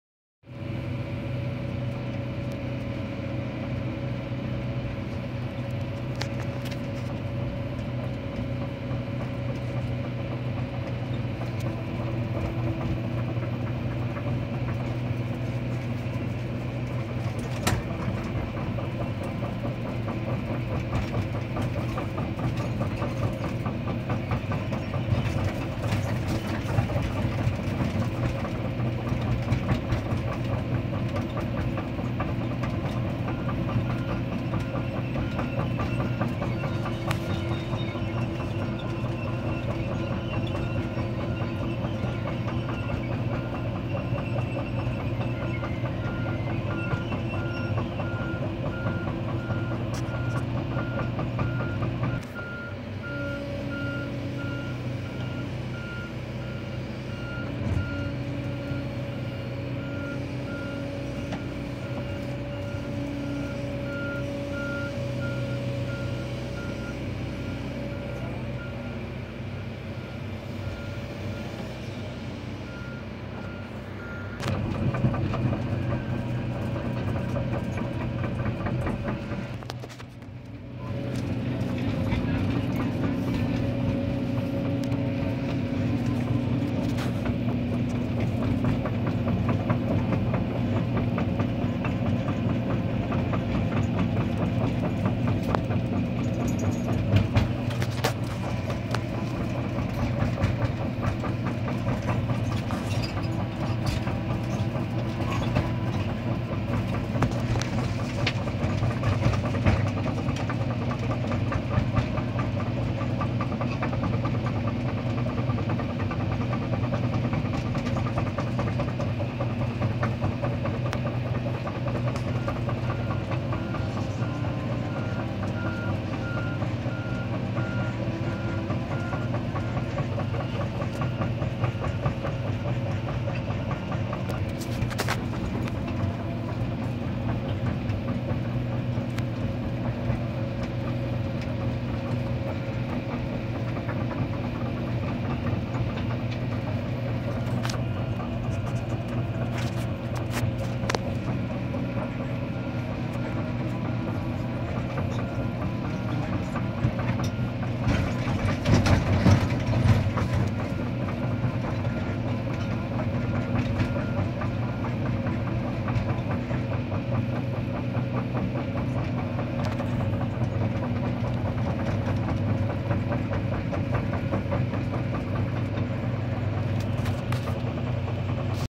Excavator kobelco sk 200 10
Excavator kobelco Langsir batu untuk buat jalan berlumpur

motor keruk, excavator besar, cara gali batu pake excavator bucket